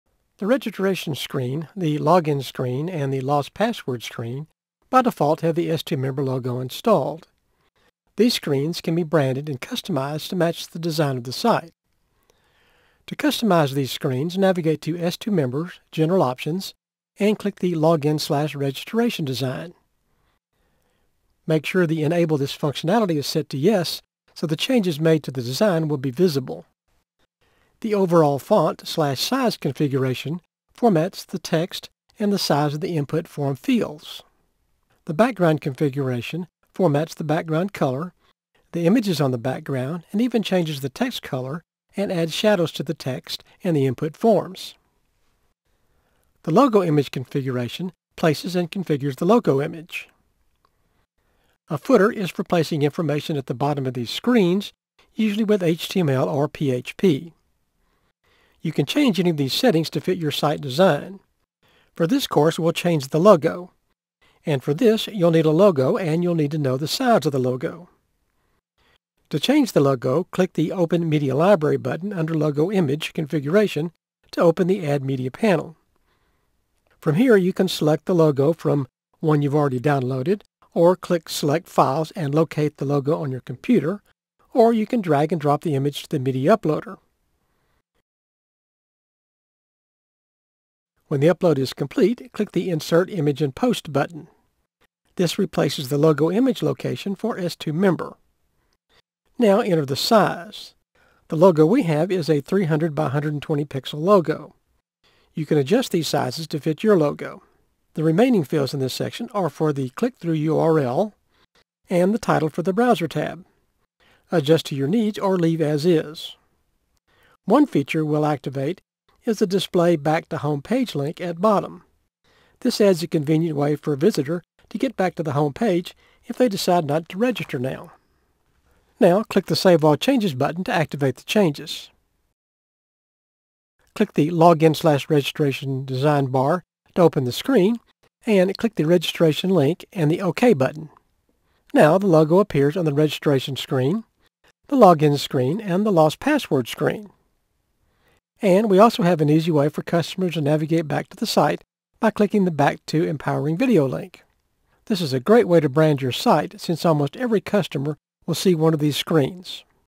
S2Member Registration Page
Create a Membership Website with WordPress - The registration screen, the login screen and the lost password screen by default have the S2Member logo installed. These screens can be branded and customized to match the design of the site.

To customize these screen s navigate to S2Members General Options and click Login/Registration design. Make sure the Enable This Functionality is set to yes so the changes made to the design will be visible.

The Overall Font / Size Configuration formats the text and the size of the input form fields.

The Background Configuration formats the background color, the images on the background and even changes the text color and add shadows to the text and the input forms.

The Logo Image Configuration places and configures the logo image.

And footer is for placing information at the bottom of these screens usually with HTML or PHP.

You can change any of these settings to fit your site design. For this course we'll change the logo. And for this you'll need a logo and you'll need to know the size of the logo.

To change the logo click the Open Media Library button under Logo Image Configuration to open the Add Media panel.
From here you can select the logo from one you've already downloaded or click Select files and locate the logo on your computer. Or you can drag and drop the image to the media uploader. When the upload is complete click the Insert Image in Post button.

This replaces the Logo Image Location for S2Member.

Now enter the size. The logo we have is 300 x 120 pixels. You can adjust these sizes to fit your logo.

The remaining fields in  this section are for the click through URL and the Title for the Browser tab. Adjust to your needs or leave as is.

One feature we will activate is the Display Back to Home Page Link at Bottom. This adds a convenient way for a visitor to get back to the home page if they decide not to register now.

Now click the Save All Changes button to active the changes.

Click the Login/Registration Design bar to open the screen and click the Registration link and the OK button.

Now the logo appears on the registration screen, the Login screen and the Lost Password screen. And we also have an easy way for customers to navigate back to the site by clicking the Back to Empowering Video link.

This is a great way to brand your site since almost ever customer will see one of these screens.